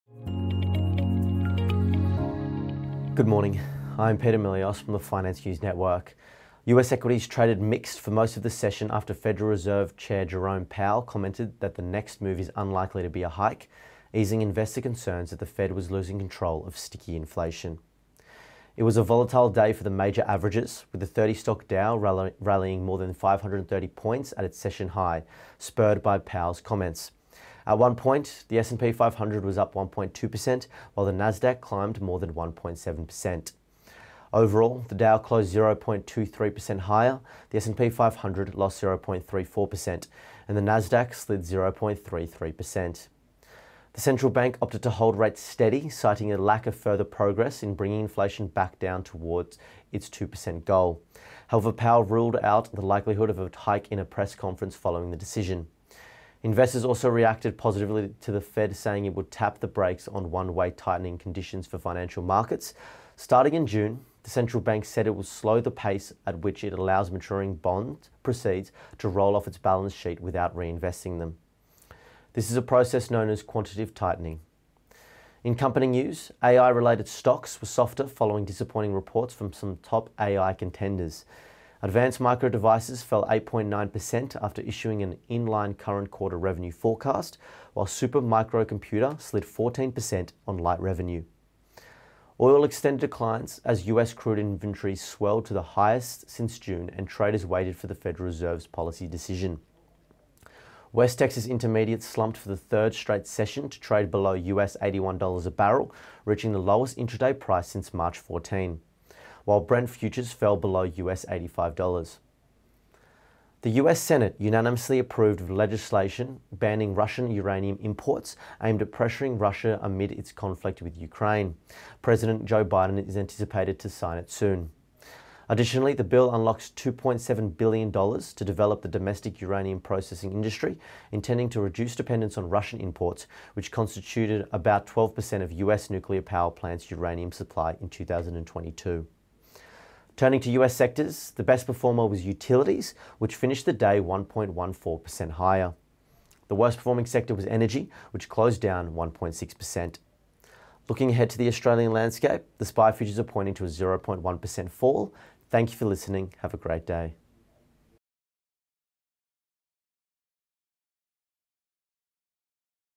US markets mixed as Fed opts to hold rates steady
US equities traded mixed for most of the session after Federal Reserve Chair Jerome Powell commented that the next move is unlikely to be a hike, easing investor concerns that the Fed was losing control of sticky inflation.